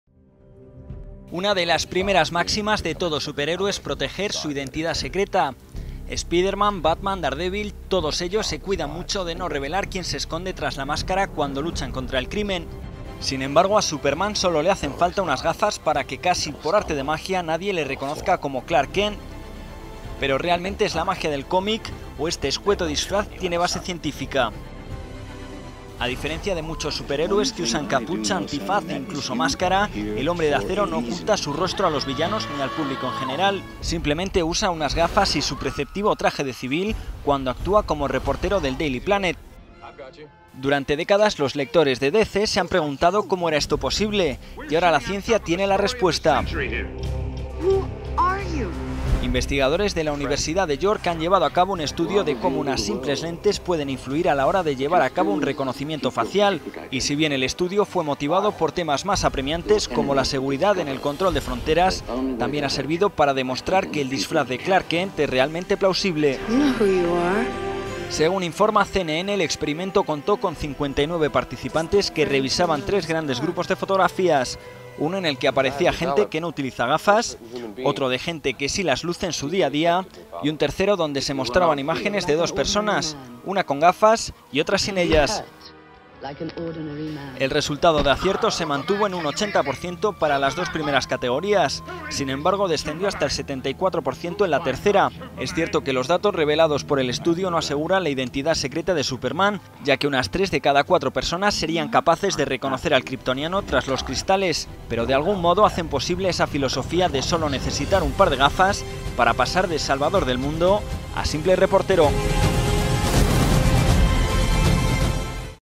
Científicos cuestionan el disfraz de Clark Kent
Una de las primeras máximas de todo superhéroe es proteger su identidad secreta. Spiderman, Batman, Daredevil... Todos ellos se cuidan mucho no revelar quién se esconde tras la máscara cuando luchan contra el crimen. Sin embargo, a Superman sólo le hacen falta unas gafas para que, casi por arte de magia, nadie le reconozca como Clark Kent. Pero, ¿Realmente es la magia del cómic... o este escueto disfraz tiene base científica?